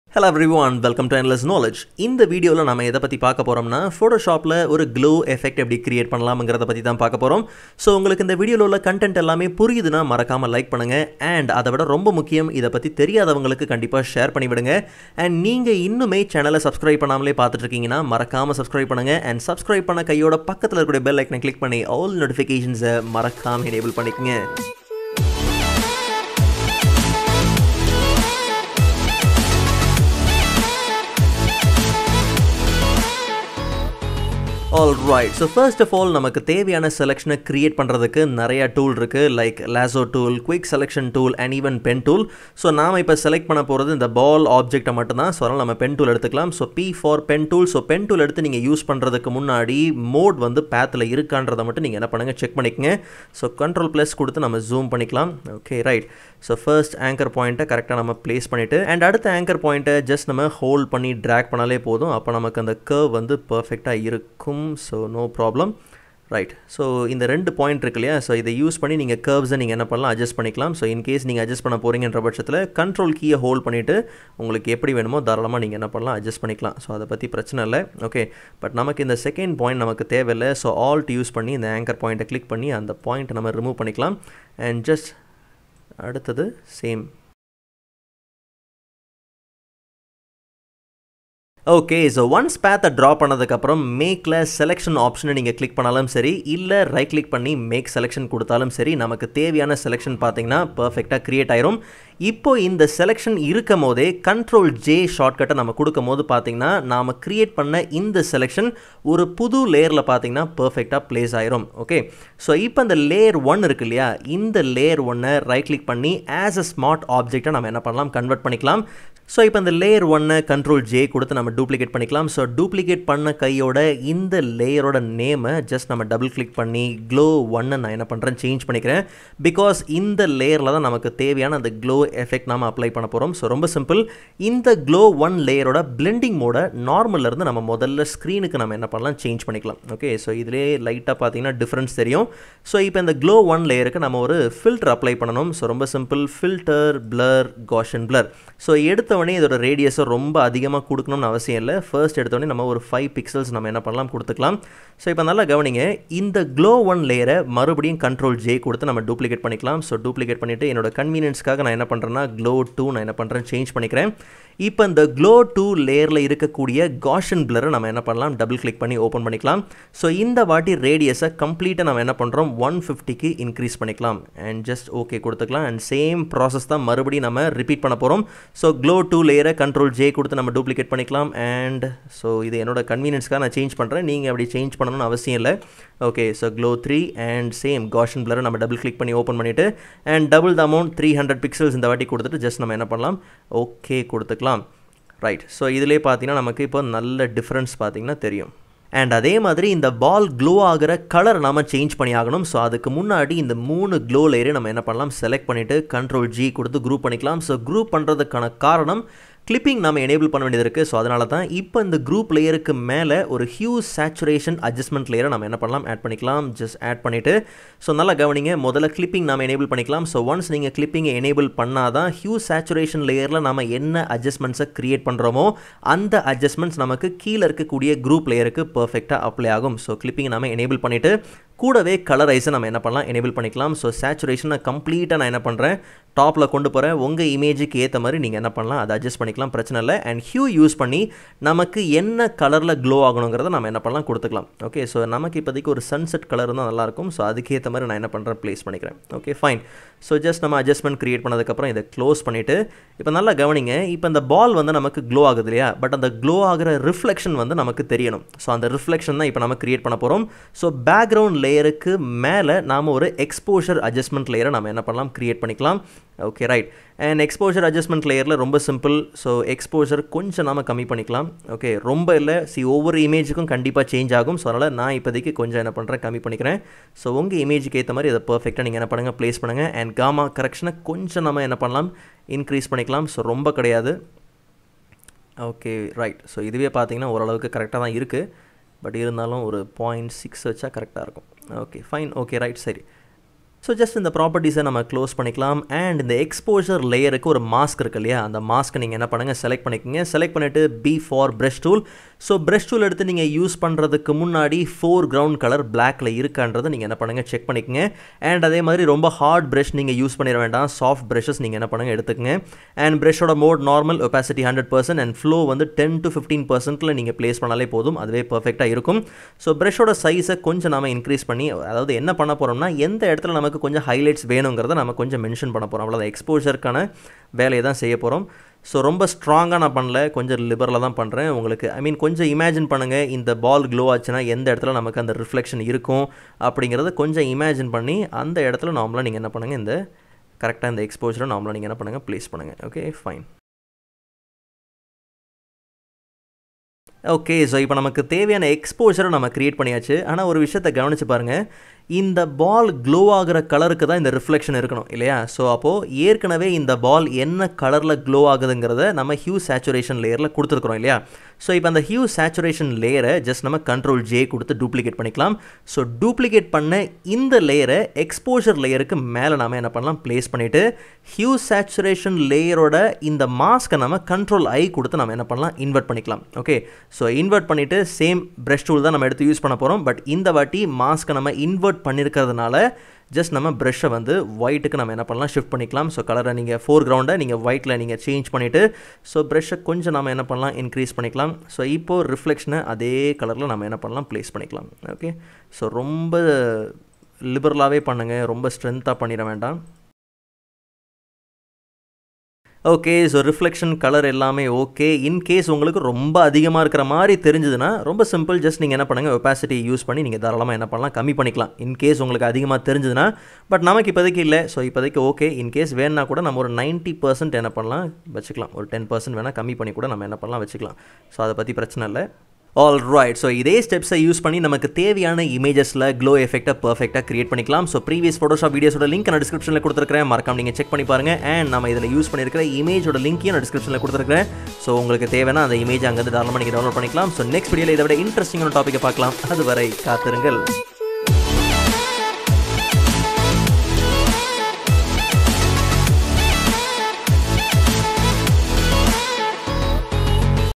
Glow Effect in Photoshop in Tamil
How to Glow Object in Photoshop step by step Tutorial.